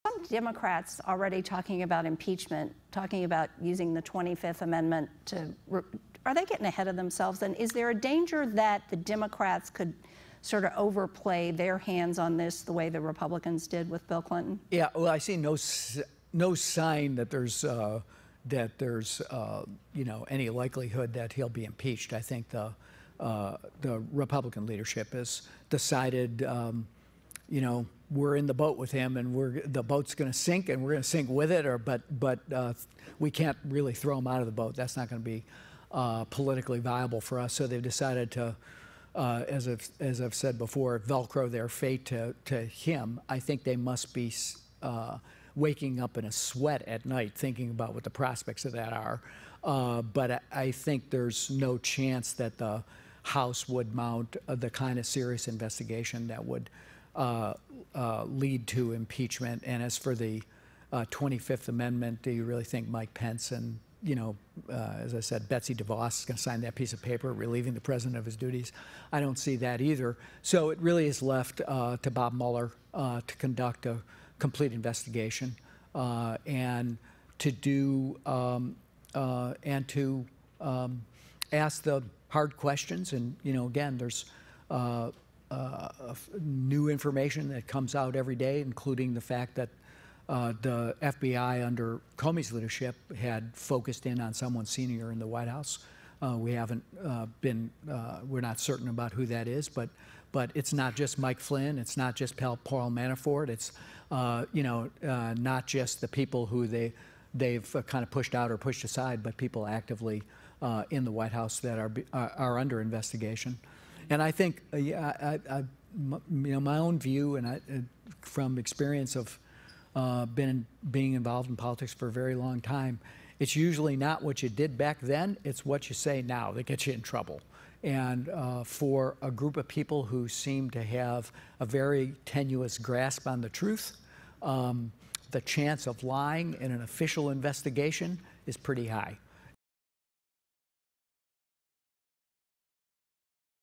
John Podesta: ‘I see no sign’ that Donald Trump will be impeached
John Podesta says that he sees “no chance that the House would mount the kind of serious investigation” that would lead to the impeachment of Donald Trump. He says that Republican leaders have decided to “velcro their fate” to the president.